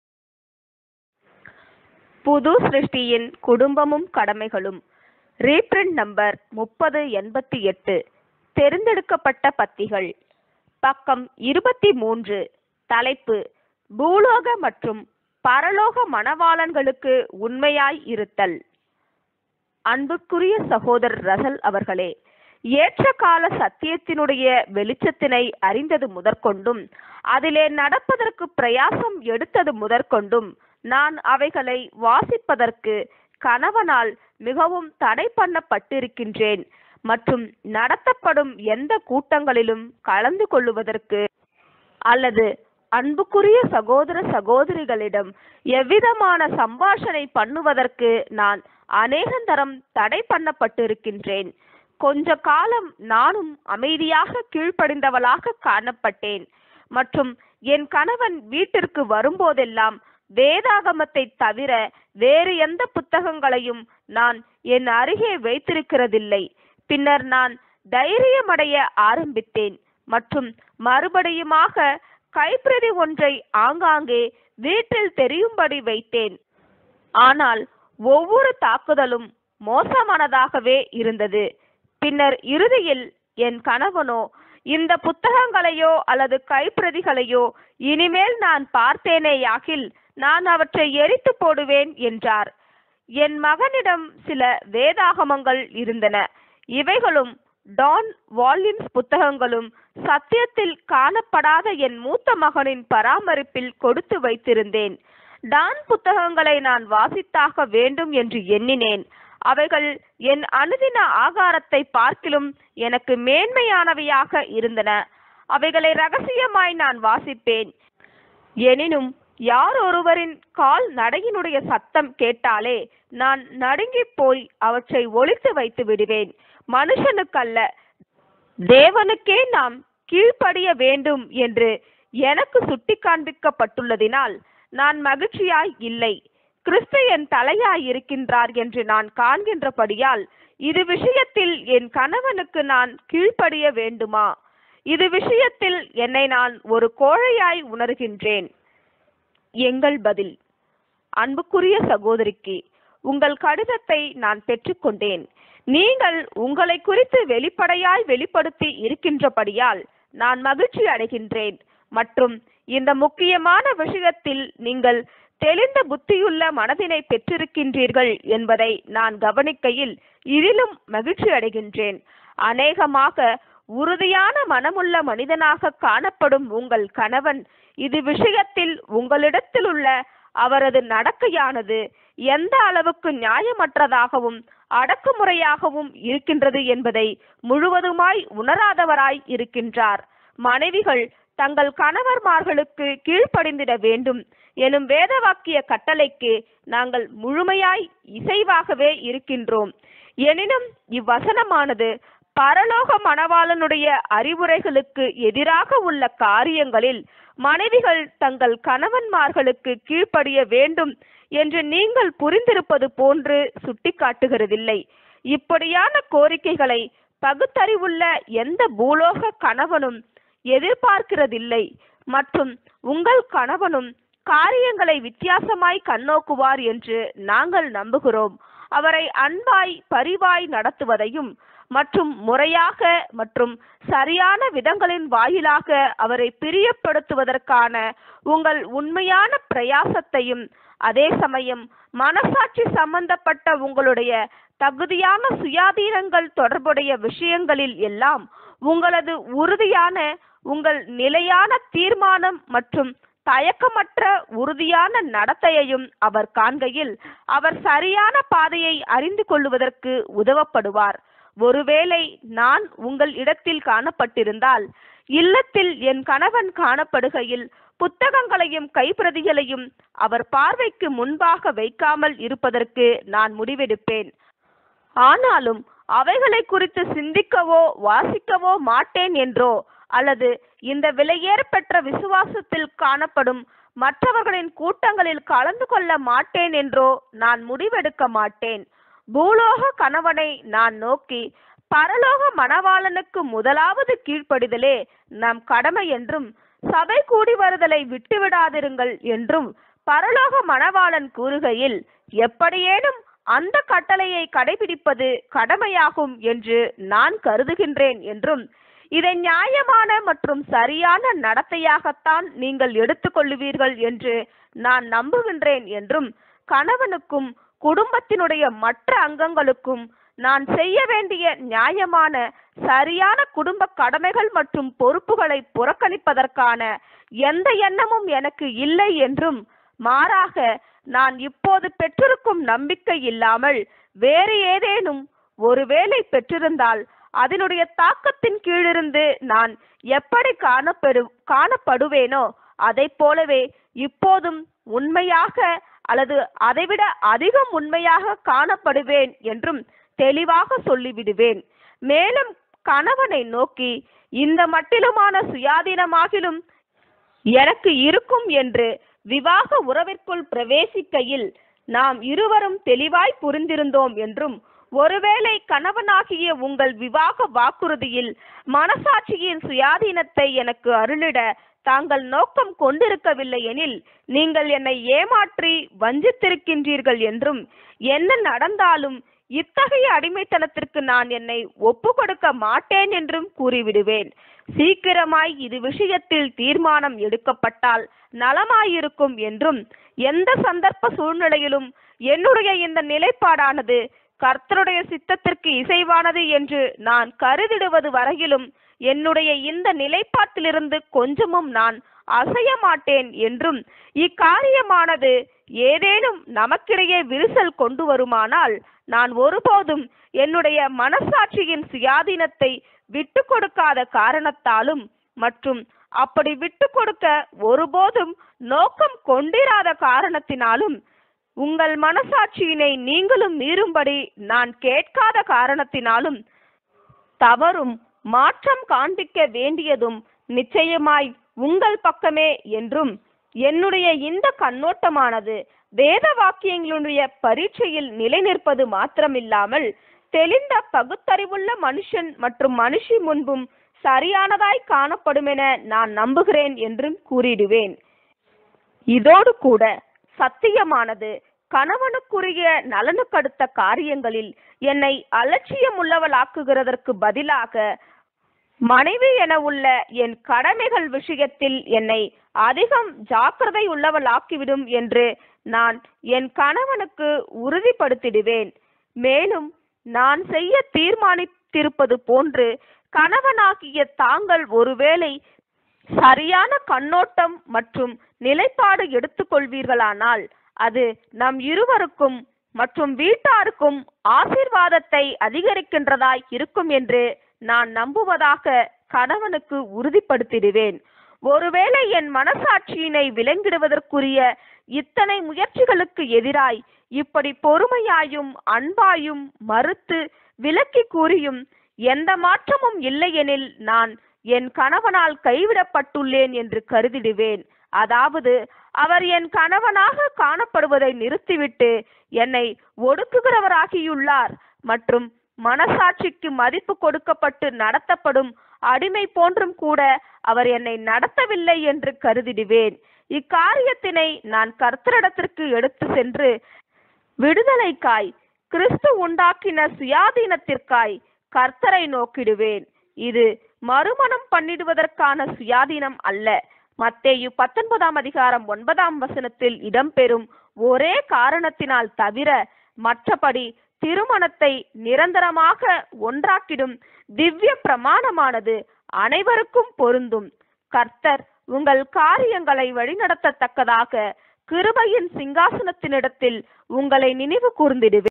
🎥 Audio (Tamil) The Family & Obligations Of New Creation R3088 FIDELITY TO BRIDEGROOMS Pg23/4
🎥 Audio Recording (Tamil) 📚 The Family & Obligations Of The New Creation 📄 R 3088 - FIDELITY TO EARTHLY AND HEAVENLY BRIDEGROOMS - Pg 23/4 Free Bible Study Br Raju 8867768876